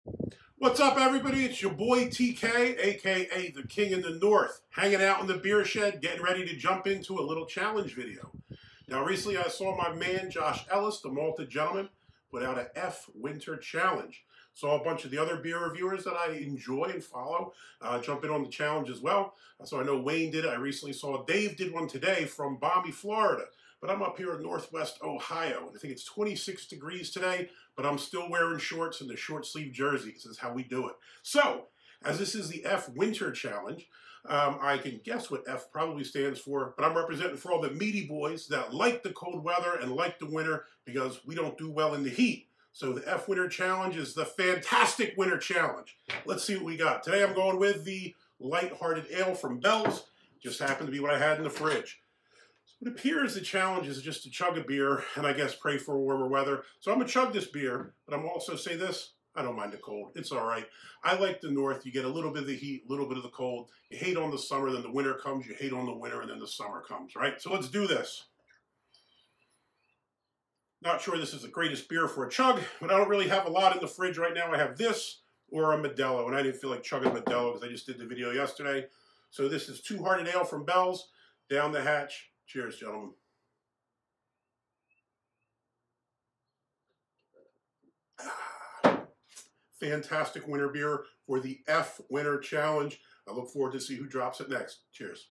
F@&# Winter Chug Challenge
The Malted Gentleman, Josh Ellis started the F Winter Chug Challenge, so I figured I would get in on the action. Check out Josh's channel and some of the other great beer reviewers who participated in the challenge, including Blue_Ice, Wayne's Beer Delivery and Dave's TV.